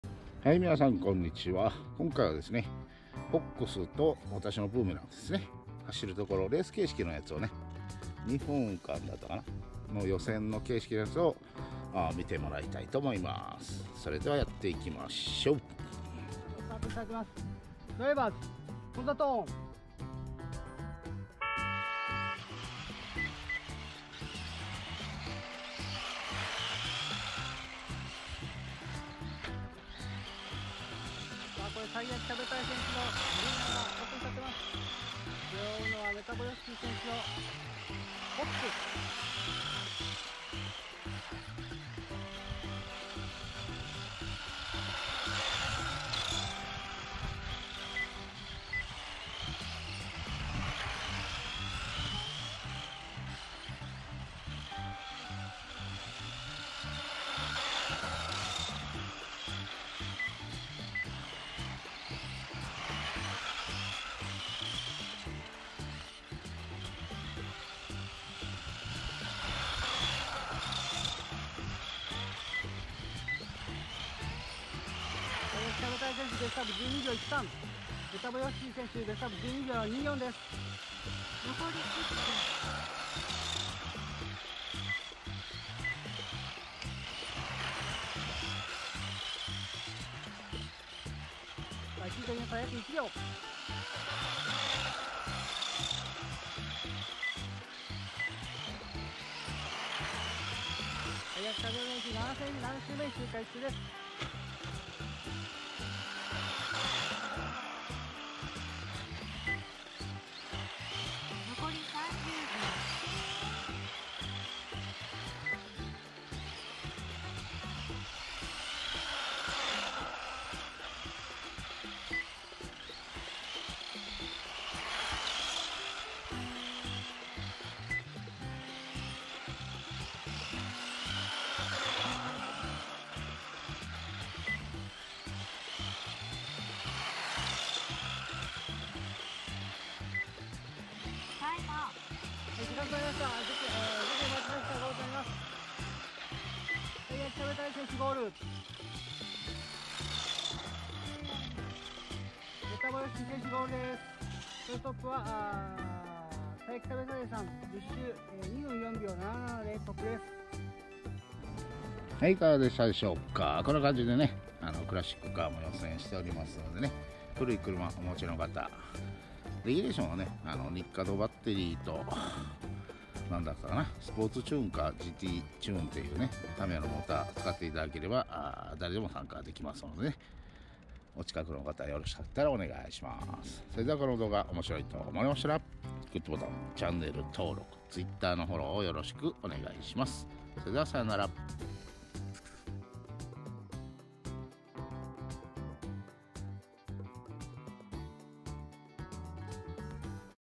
【ラジコン】FOX vs BOOMERANG /フォックスとブーメランでレースの予選形式で走ってみました❗️
2分間の予選形式で走りました。